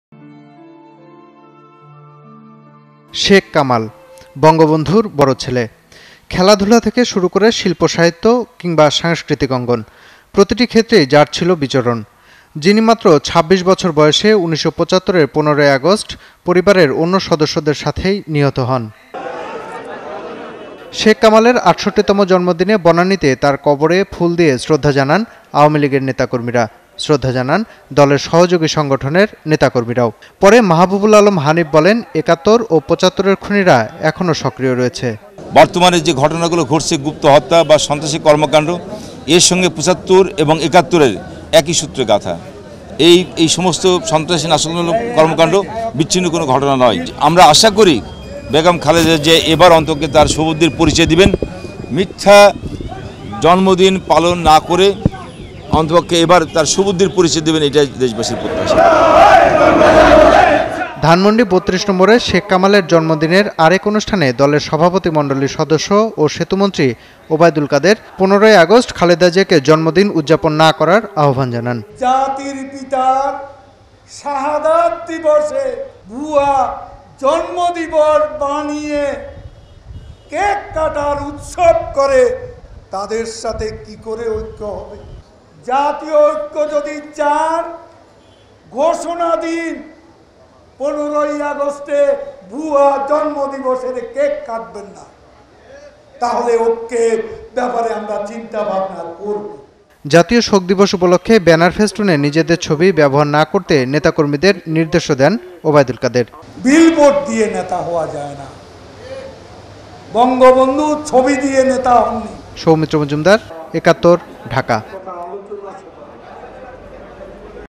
Soumitra Mazumdar Sheikh Kamal PKG
A story of Sheikh Kamal, elder son of slain father of the nation of Bangladesh Bangabandhu Sheikh Mujibur Rahman, who also killed with his other family members on 15 August, 1975.